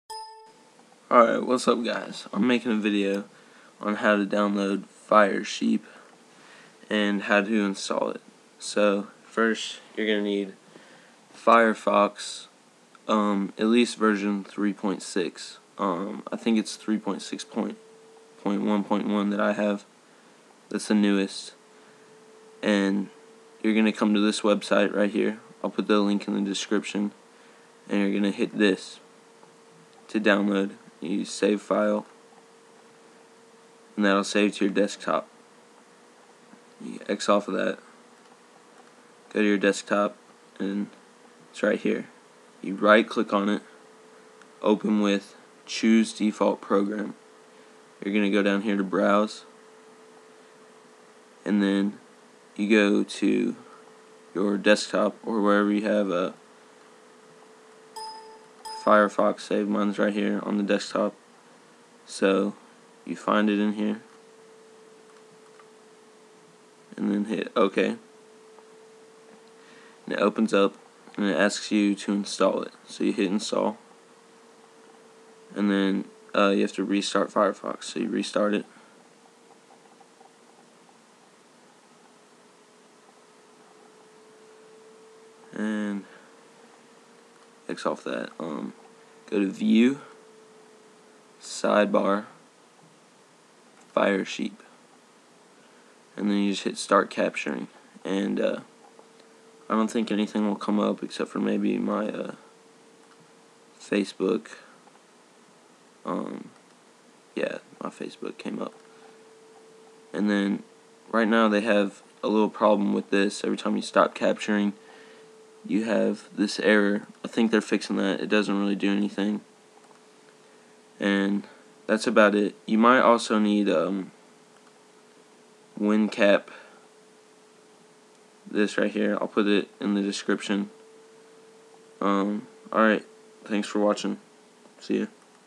How To Download And Install Firesheep
This Video Will Teach You How To Download And Install and use Firesheep. If you have trouble or have questions leave a comment or PM me!

I know this is general but i can help with specific problems you may have.